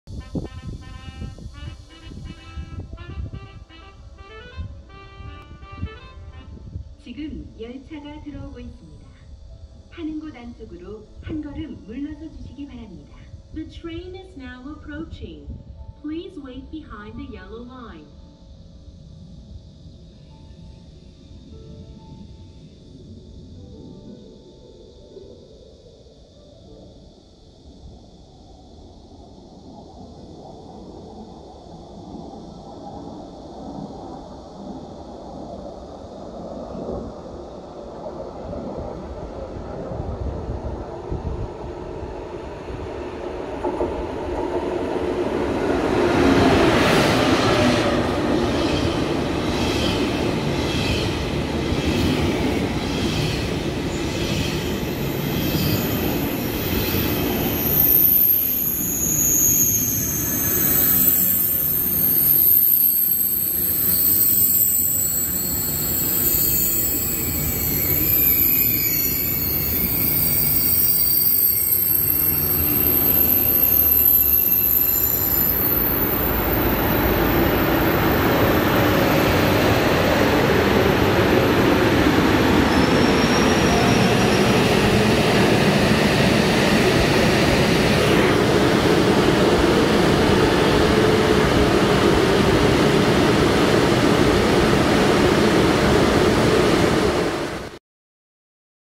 여수EXPO행 KTX #509 전주역 진입(015호기)
2021년 7월 28일 촬영

여수EXPO행 KTX 열차 전주역 진입

The KTX train for Yeosu EXPO, approaching Jeonju Station